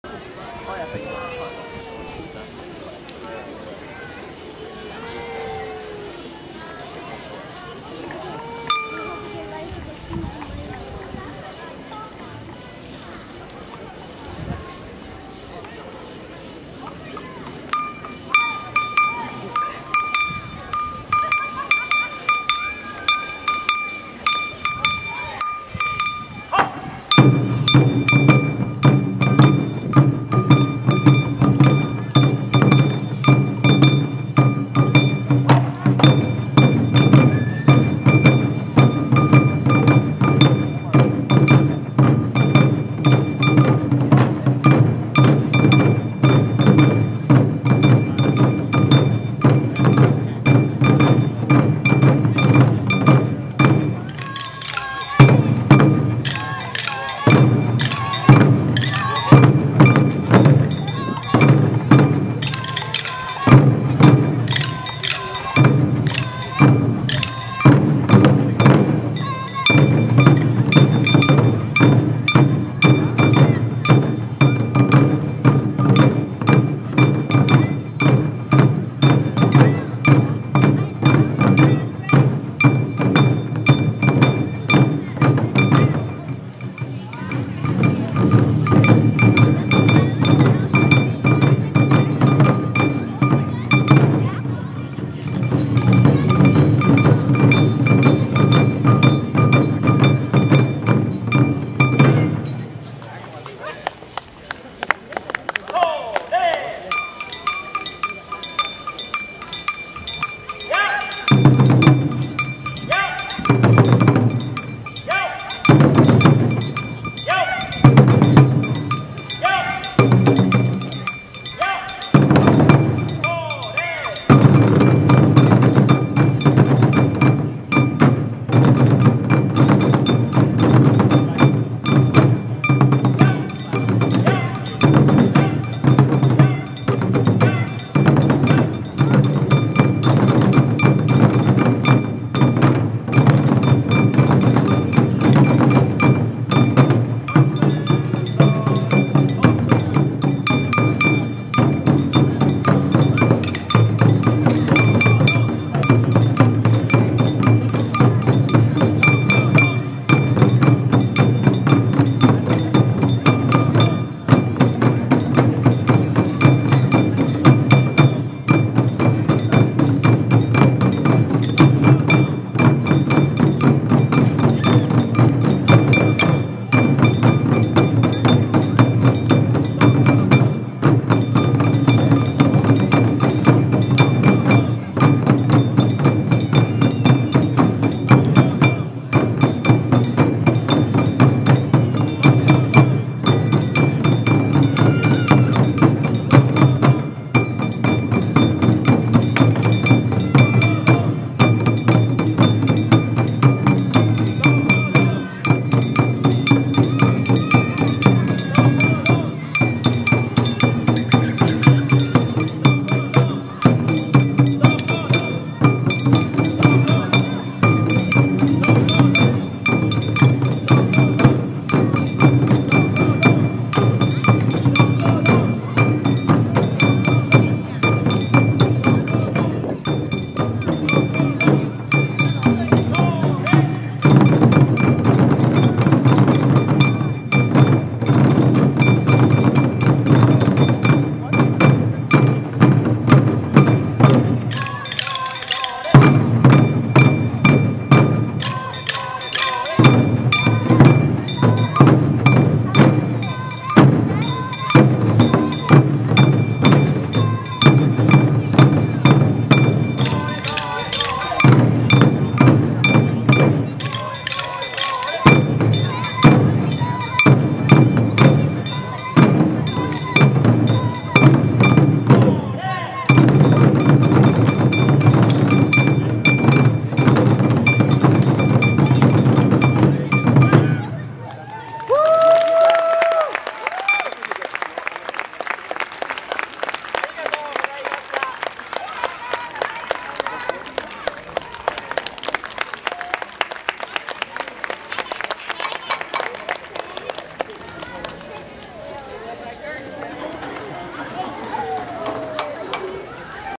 2013.10.27.4 子供囃子
It is the performance of TK Taiko Drum Class of the Yokosuka  CommunityCenter.
It is a class of third(Taiko Intermediate).